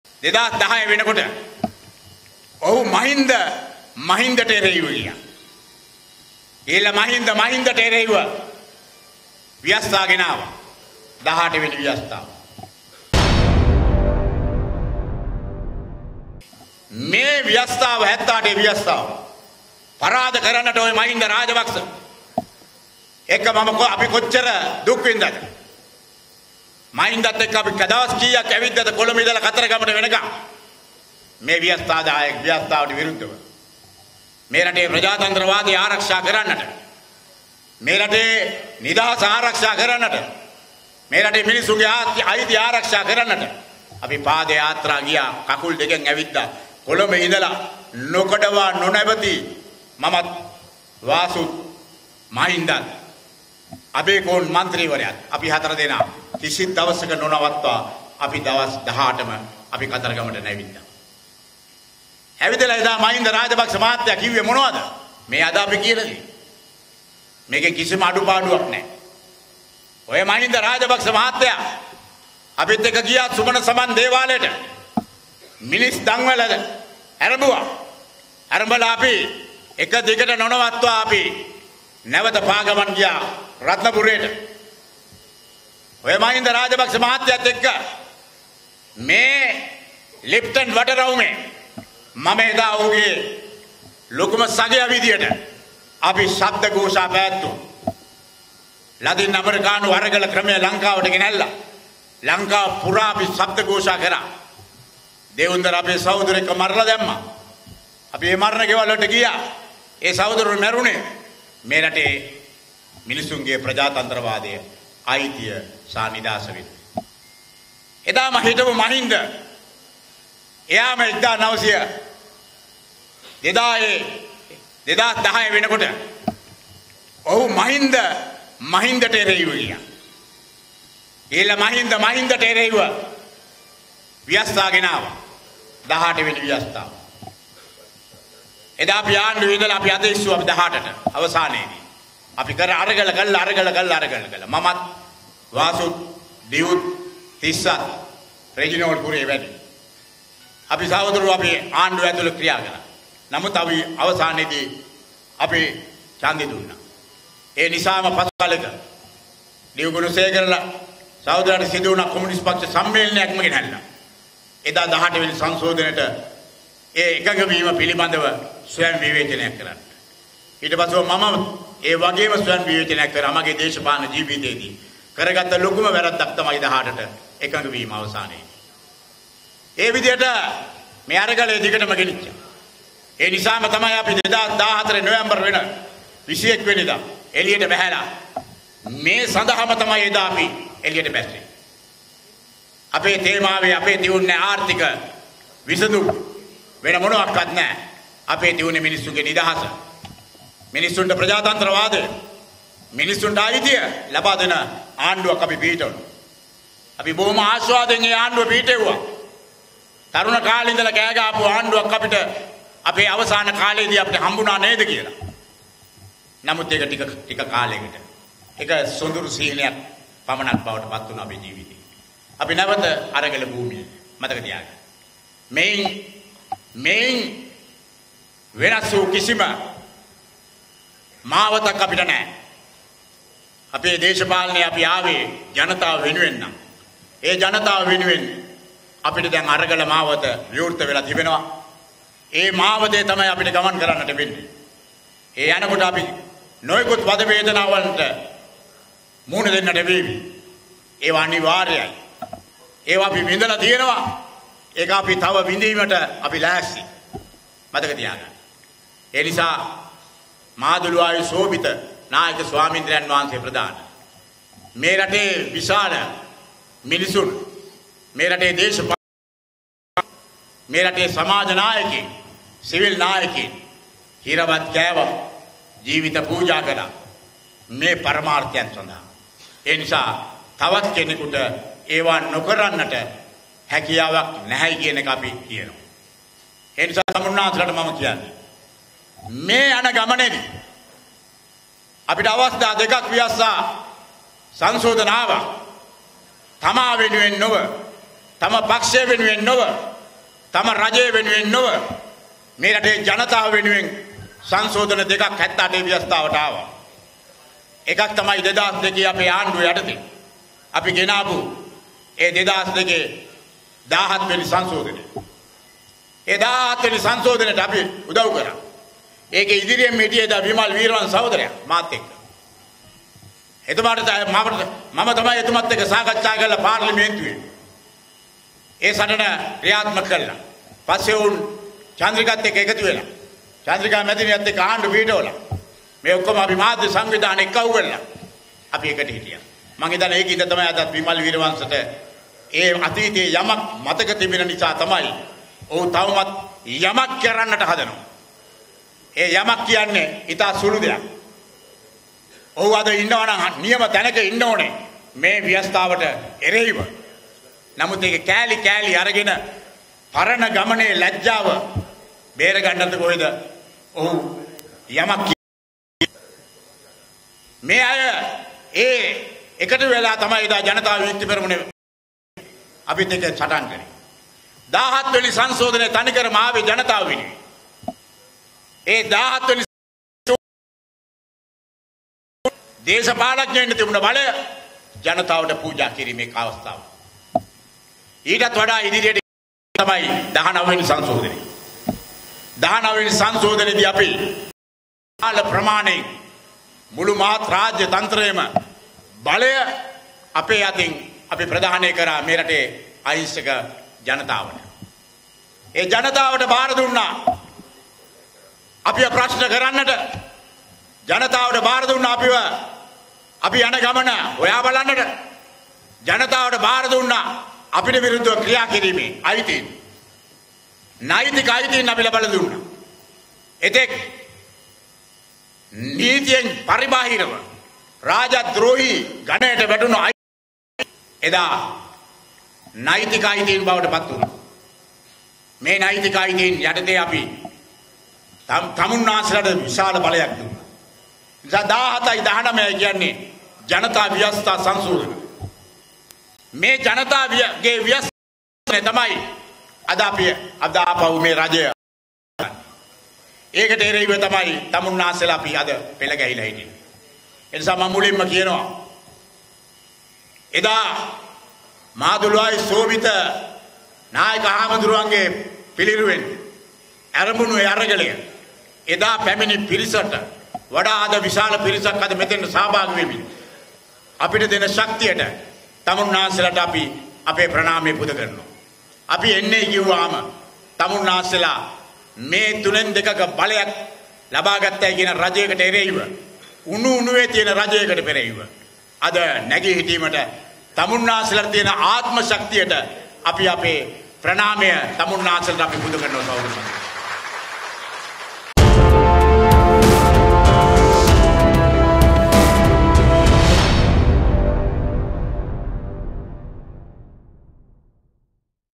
Rajitha Senarathna Questioned Why Mahinda Walk To Katharagama Against The Constitution| Apuru Gossip
Samagi Jana Balwegaya Parliamentarian Rajitha Senarathna Questioned Why Mahinda Walk To Katharagama Against The Constitution.